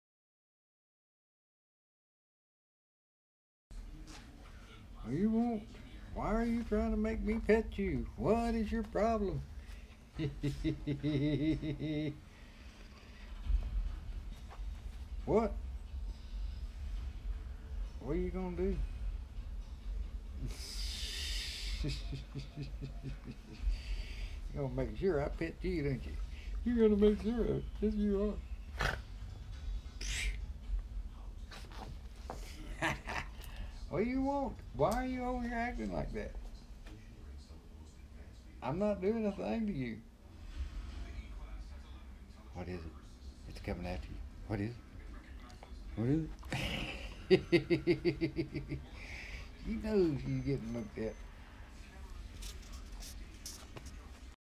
Tu-Tu - Attention hog!
My pup, making sure she gets her lovins!